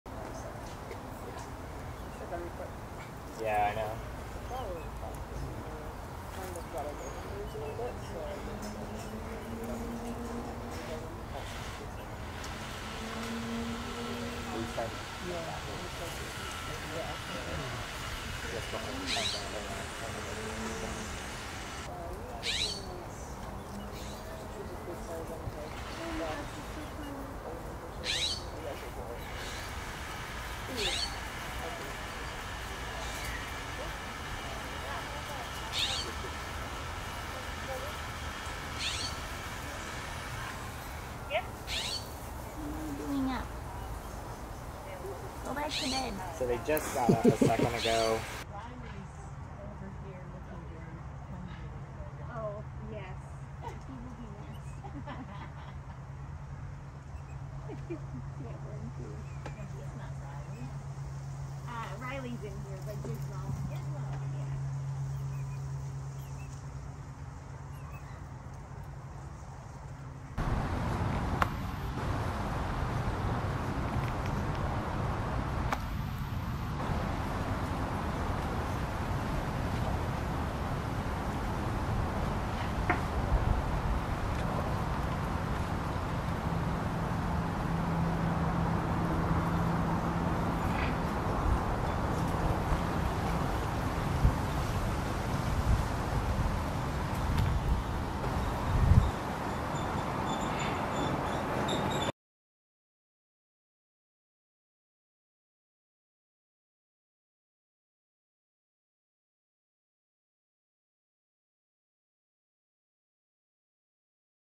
Deer Me, That's One Adorable Baby Pudu!
Oakland Zoo’s pair of Southern Pudu have welcomed a fawn to their family -- a male, born August 7.

The following day, Vet and Animal Care staff did an exam and found that he is doing well. They’ve observed the fawn nursing on several occasions, and he has been taking very short trips around the habitat with Mom. Animal Care staff will continue to monitor the family closely to make sure everyone is staying happy and healthy.